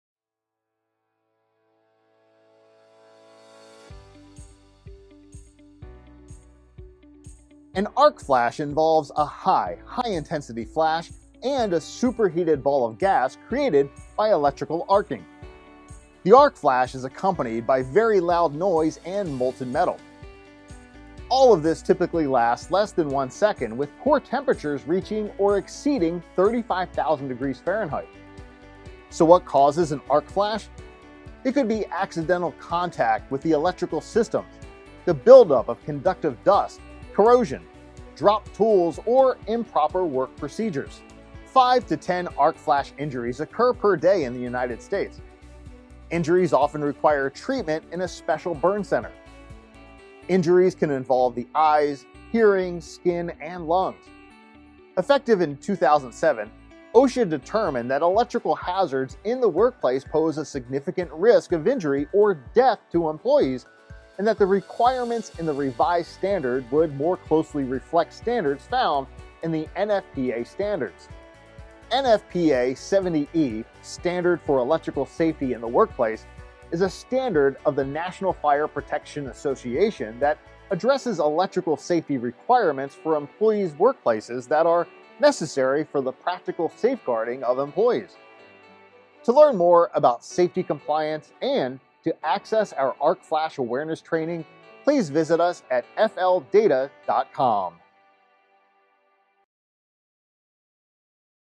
EHS Training: Arc Flash 101
Welcome to EHSTraining with Frontline!
Let's talk about arc flashes.

00:07 What is an arc flash?
00:28 What causes an arc flash?
00:40 How often do arc flashes accidents happen?
00:54 Is arc flash training required by OSHA?

About Frontline
Frontline helps companies digitize, simplify and optimize critical safety operations. to learn more.